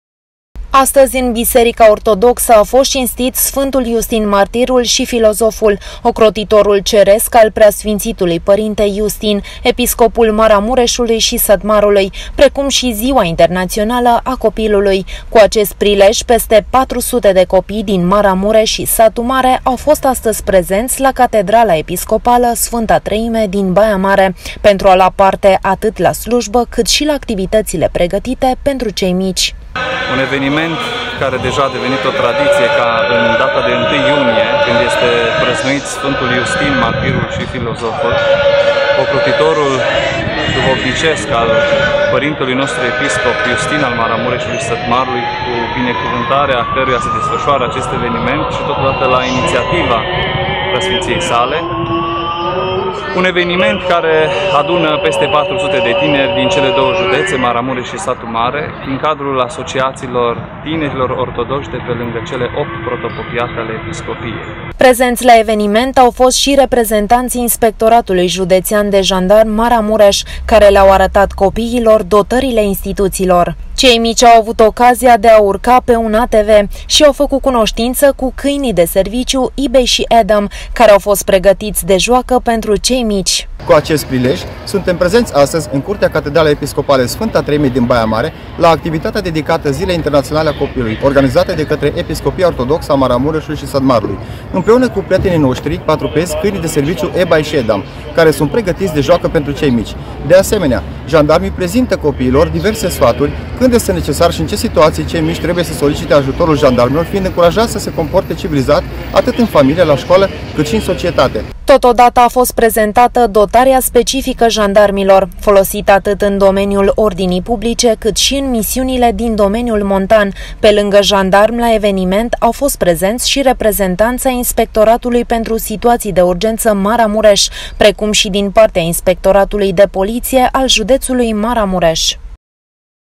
1 IUNIE, SĂRBĂTORIT SI LA CATEDRALA EPISCOPALĂ
Ziua Copilului a fost sărbătorită și la Catedrala Episcopală ”Sfânta Treime din Baia Mare”. Aici s-au adunat peste 400 tineri, atât din Maramureș, cât și din Satu Mare.